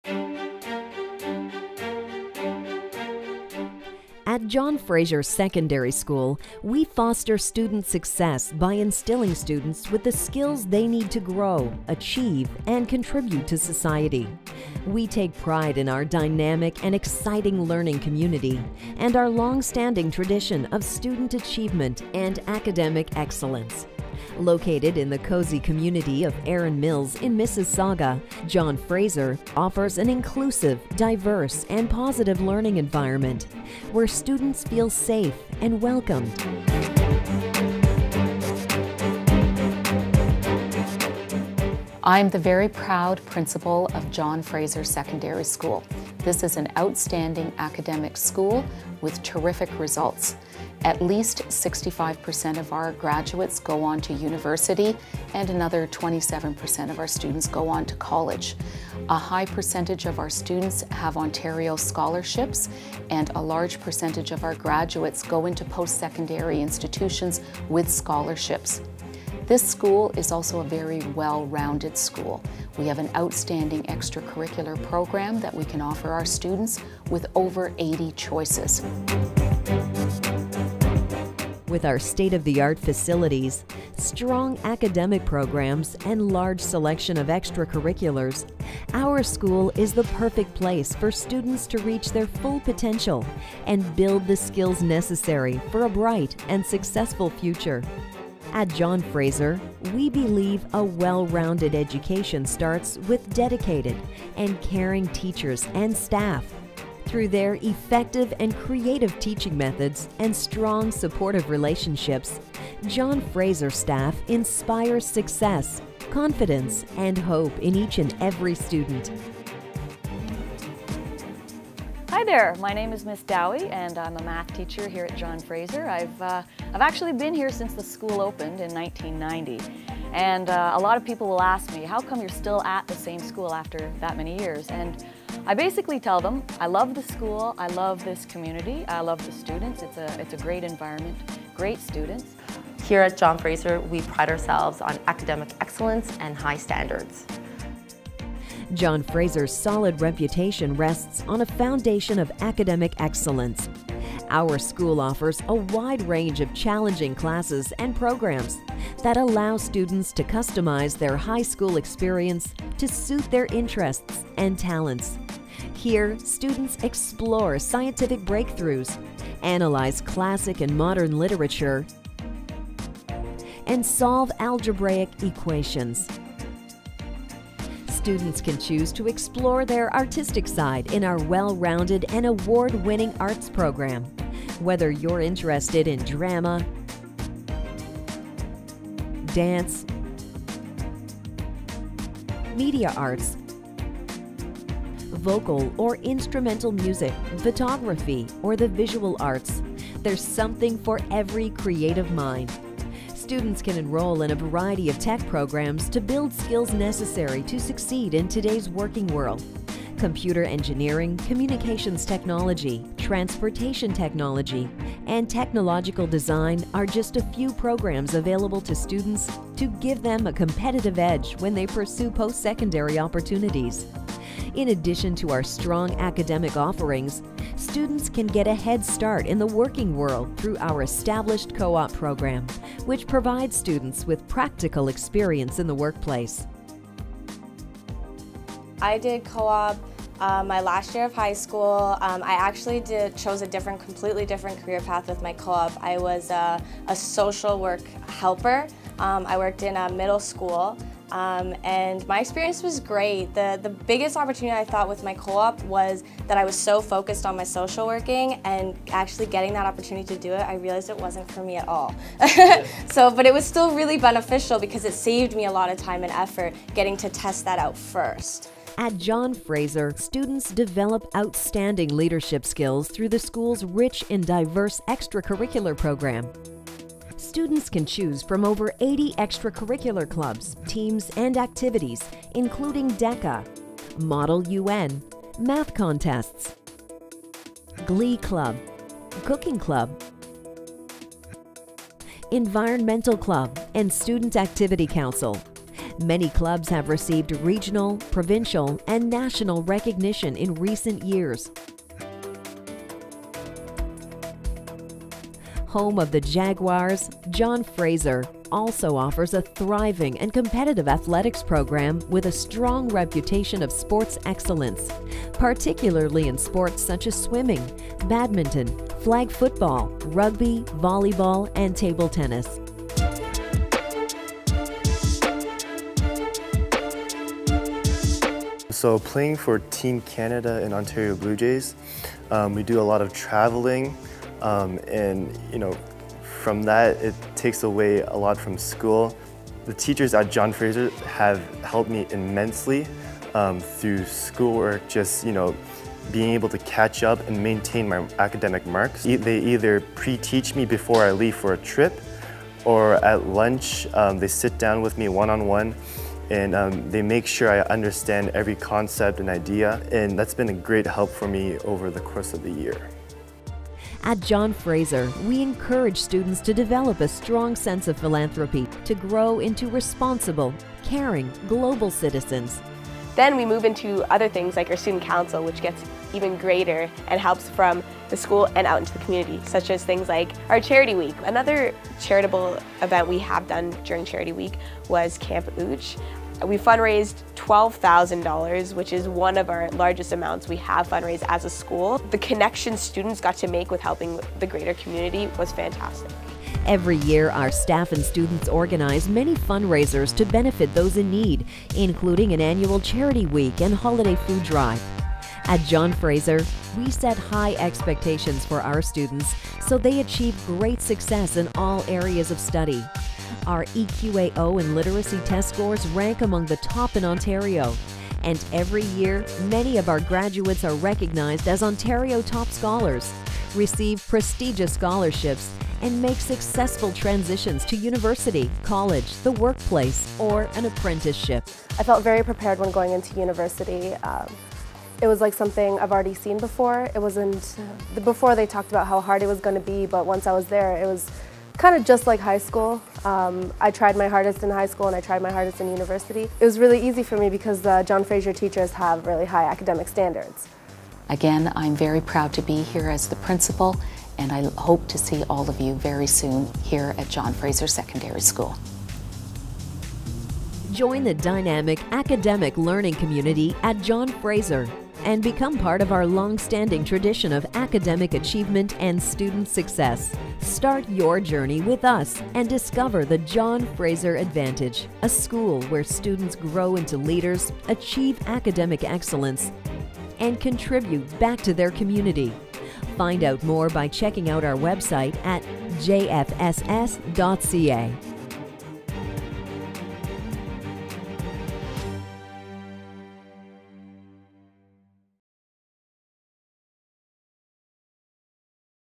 Welcome to John Fraser SS - Full version
This video takes an in depth look at what John Fraser Secondary School has to offer to new and potential students.